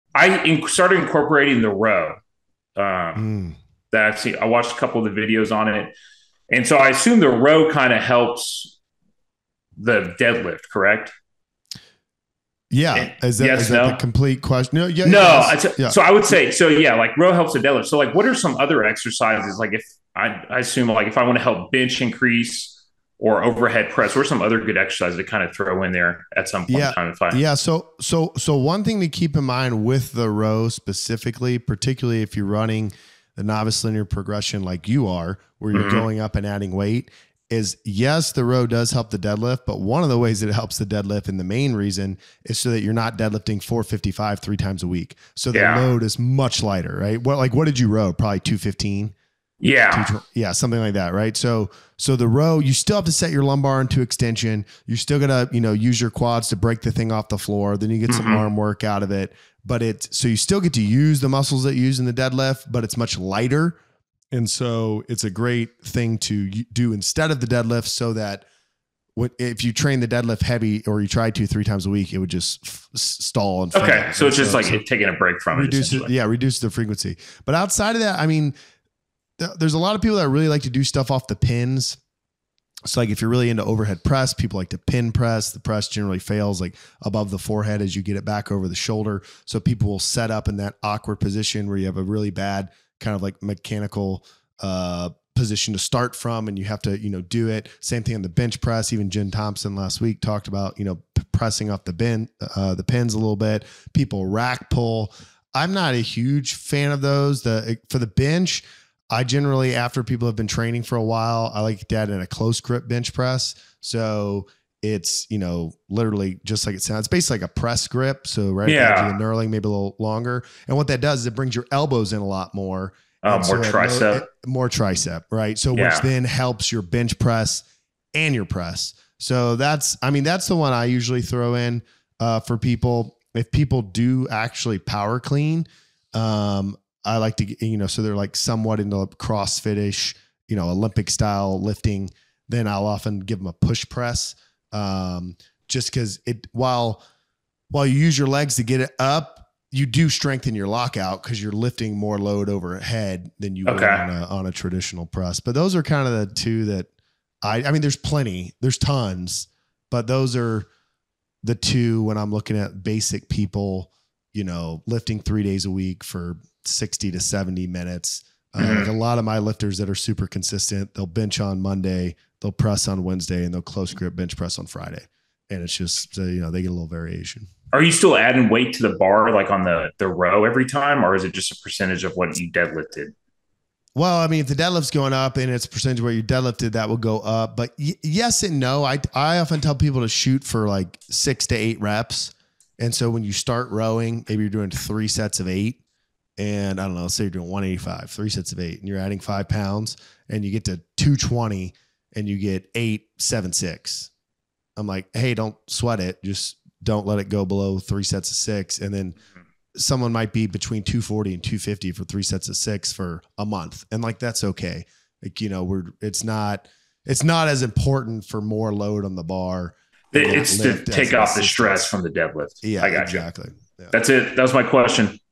Increase Your Deadlift With The Barbell Row
Starting Strength Coach Grant Broggi explains how to incorporate the barbell row into your program so that your deadlift can continue to increase.